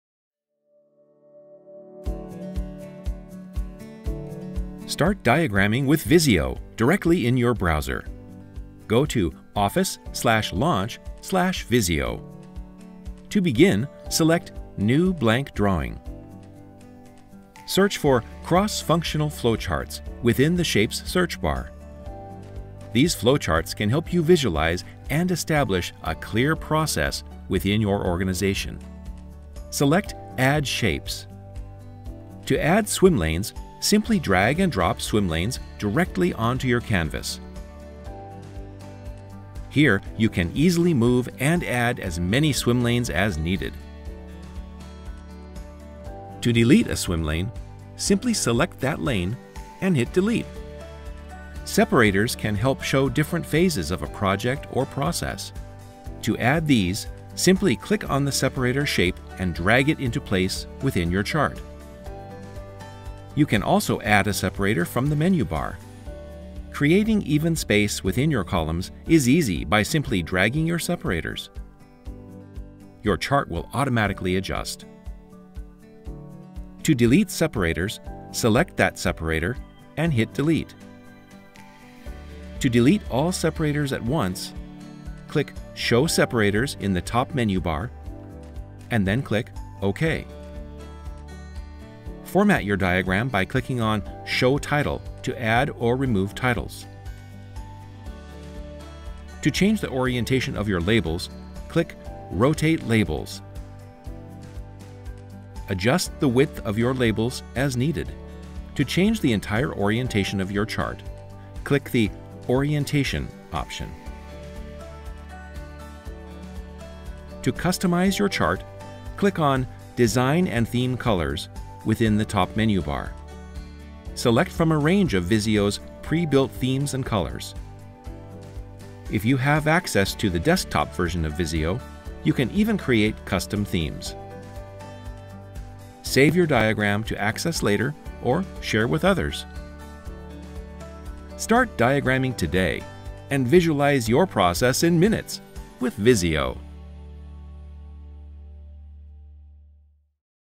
Creating swimlanes with Visio web
In this video, learn how to easily create swimlanes with Visio web. Visio makes it easy to create professional diagrams.

Get started with Visio today to learn how to create swimlanes and movers in Visio Web